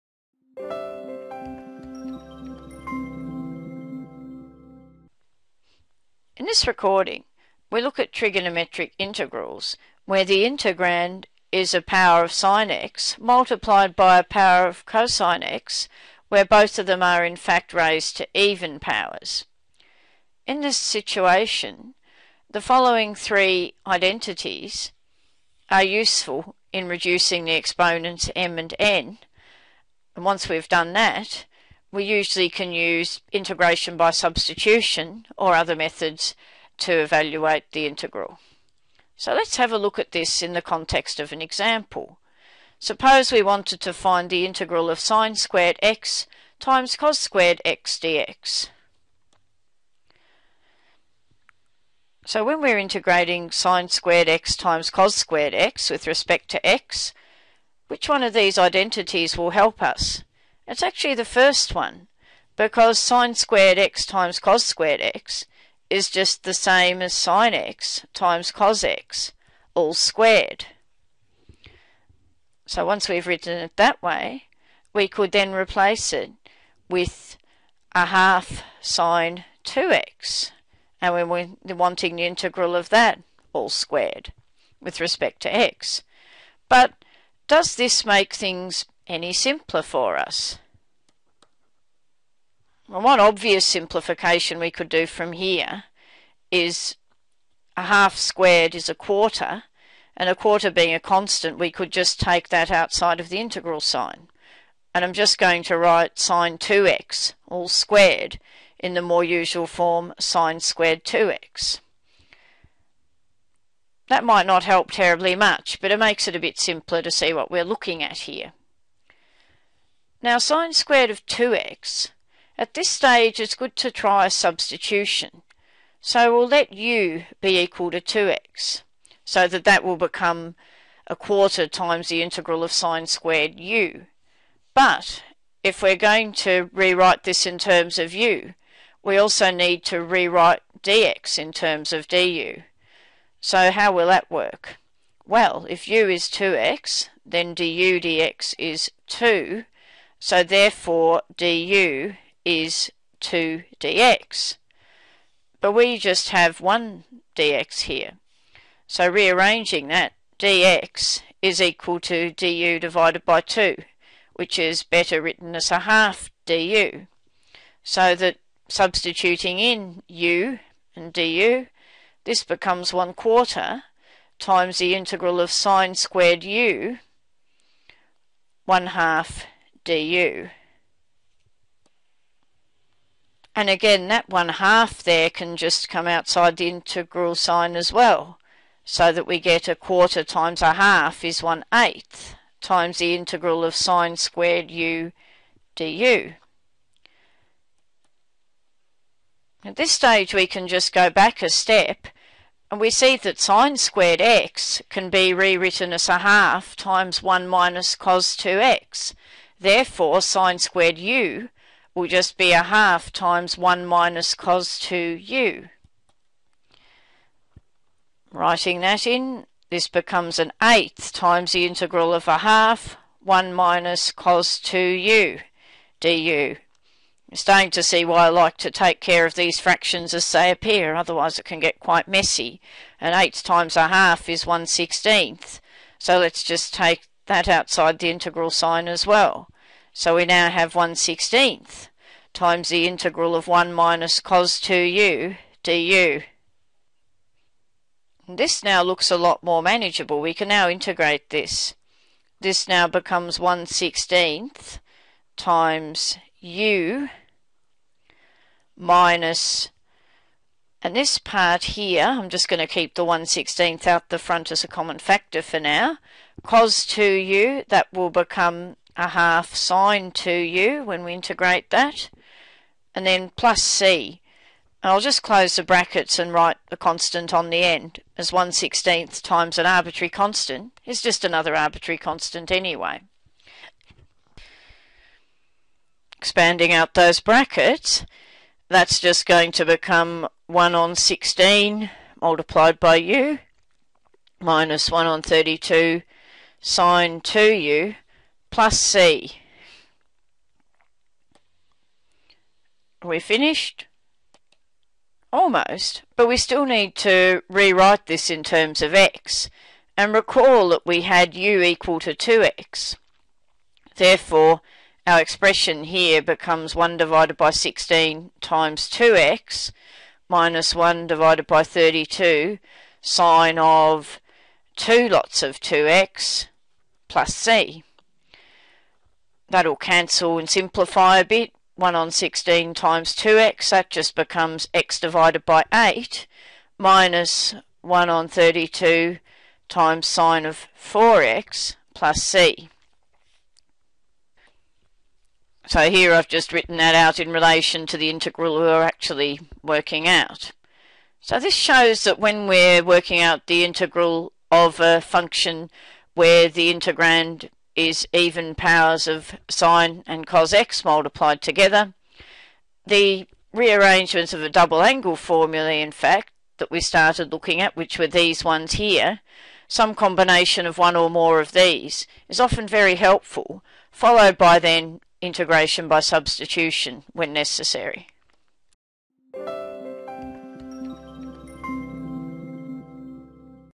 Integrals with even powers of sinx and cosx- example 1 (MathsCasts)
Demonstrates an example of integrating a function that is a product of even powers of sinx and cos x, with assistance from double-angle formulae